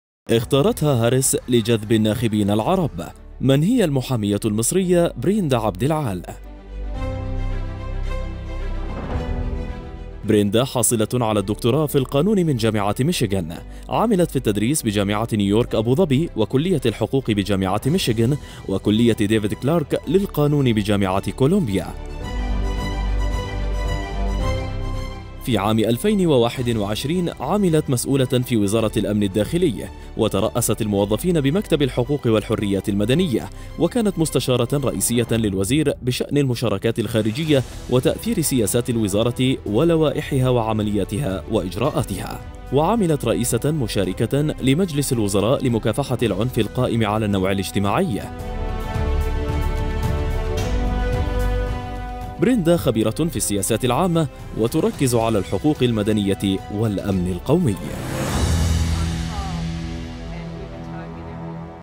اختارتها كامالا هاريس لجذب الناخبين العرب.. معلومات عن المصرية الأصل بريندا عبد العال
مسؤولة سابقة في وزارة الأمن الداخلي اختارتها كامالا هاريس لجذب الناخبين العرب.. معلومات عن المصرية الأصل بريندا عبد العال

لمعرفة أحدث المستجدات.. عبر البث المباشر من هنا
وبإمكانكم متابعة الحدث على منصات التواصل